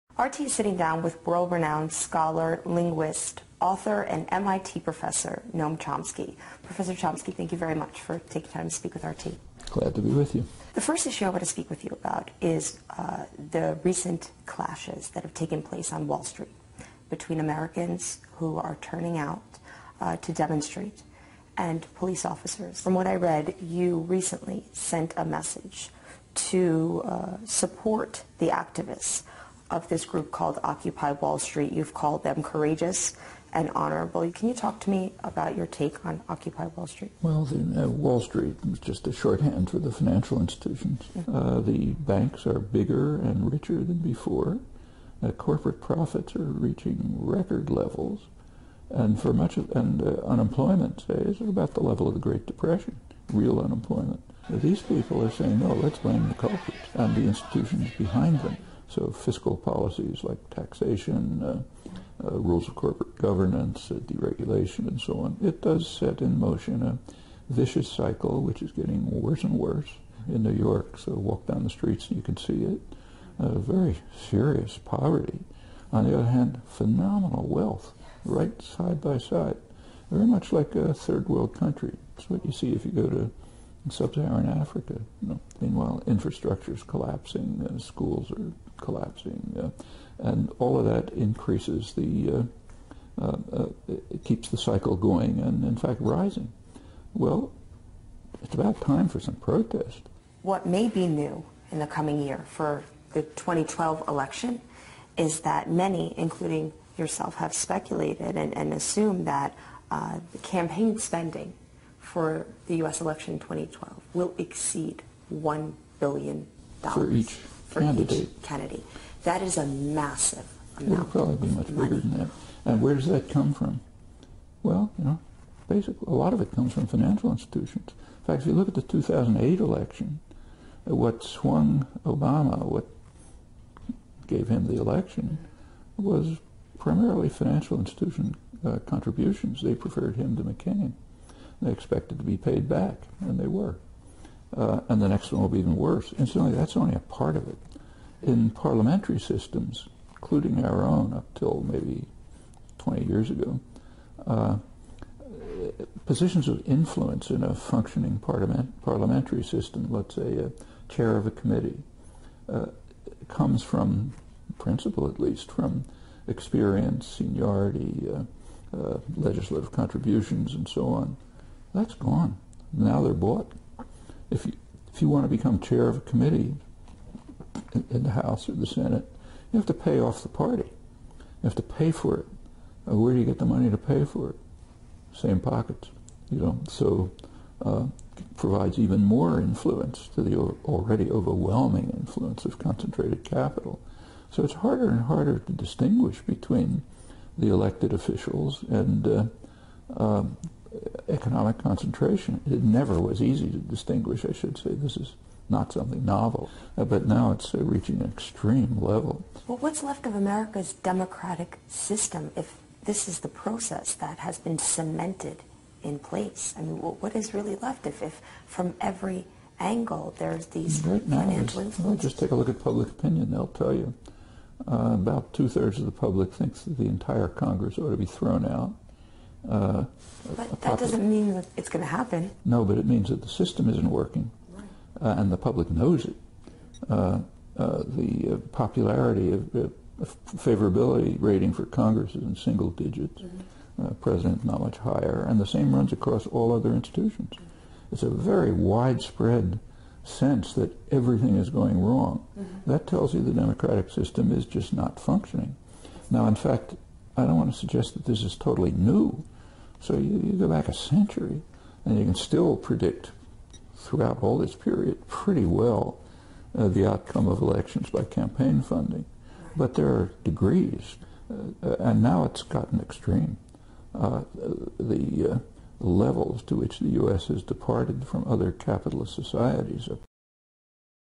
Noam Chomsky on Occupy Wall Street Corporate Insider Wallstreet Bank Fraud OWS
Noam Chomsky on Occupy Wall Street Corporate Insider Wallstreet Bank Fraud OWS  The democratic system is not functioning!
2/3d's of CONGRESSMEN should be thrown out of office!